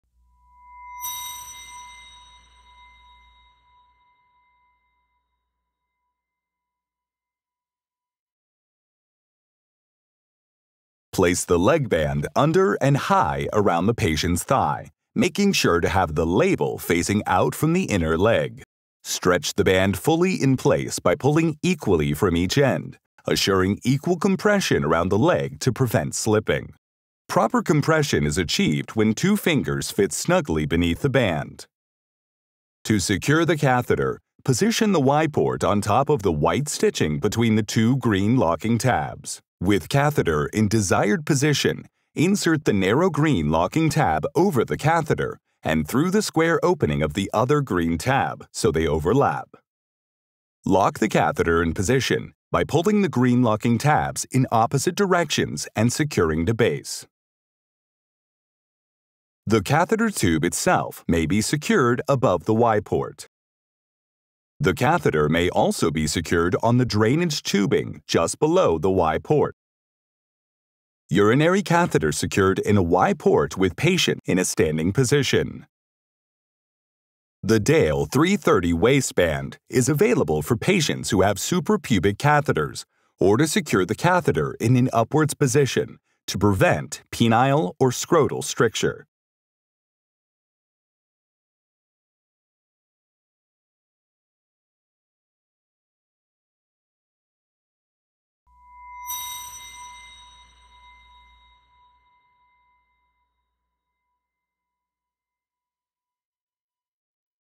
Dale Hold-n-Place® Foley Catheter Tube Holder Leg/Waist Band - How to apply
Learn the proper application of the Dale Hold-n-Place® Foley Catheter Leg/Waist Band, a comfortable solution for long-term catheterized patients that can be used with equal effectiveness on all Foley catheters including Teflon, silver and silicone coated catheters.

To learn more about the Hold-n-Place Foley Catheter Leg Band & Waist Band , view them on our